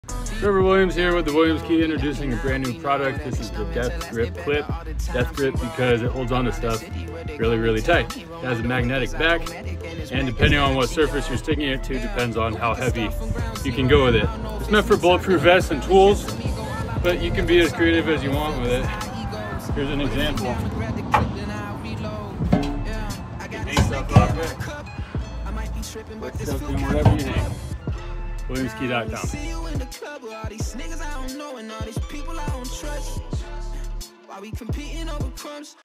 Williams Key - DEATH GRIP CLIP
New product drop - Williams Key's DEATH GRIP CLIP. HEAVY DUTY MAGNET CLIP used for numerous applications including holding up hangers, bullet proof vests & tools.